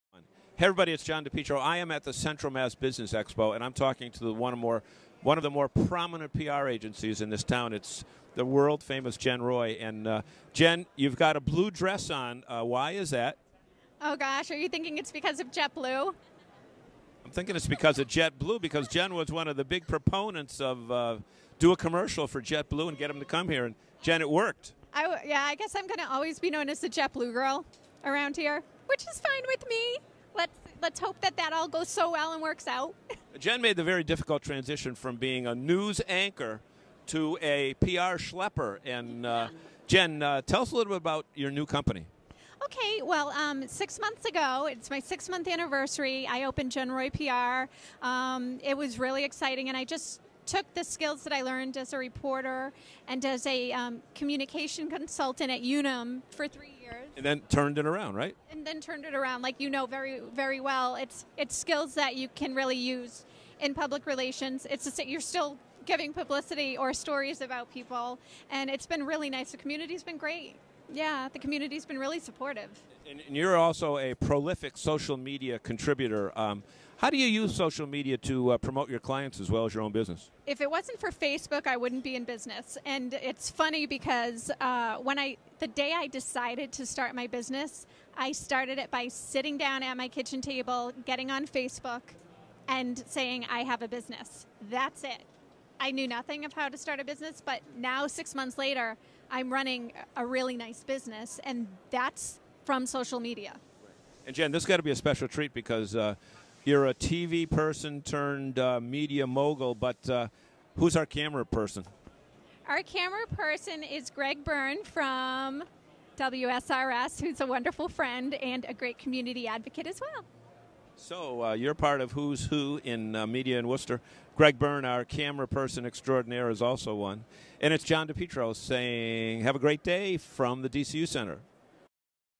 Central Mass Biz Expo Jen Roy
Here is JEN ROY of JEN ROY PR talking to John DiPietro at the Central Mass Business Expo